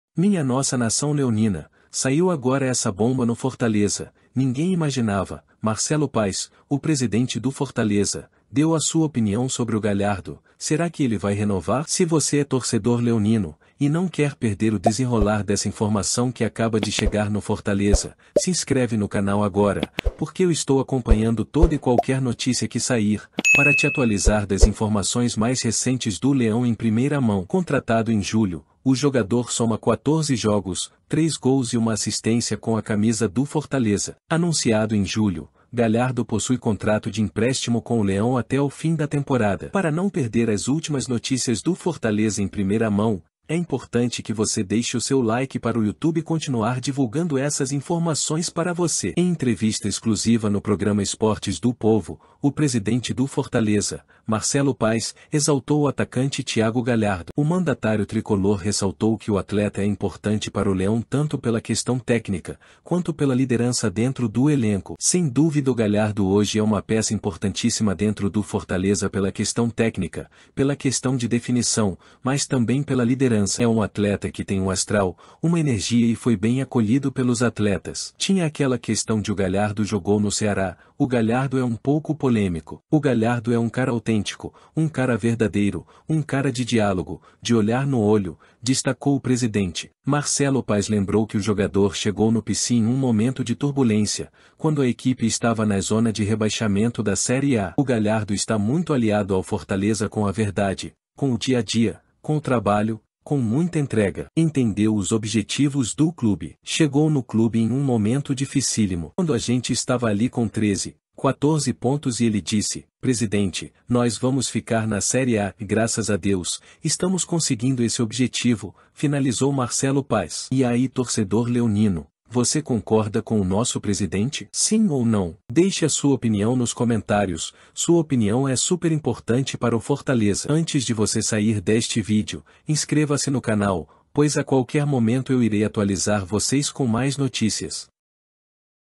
🚨MINHA NOSSA! VEJA O QUE O MARCELO PAZ FALOU DO GALHARDO! NINGUÉM ESPERAVA! NOTÍCIAS DO FORTALEZA
00:00 seja bem vindo
00:10 deixa o like
00:20 se inscreva no canal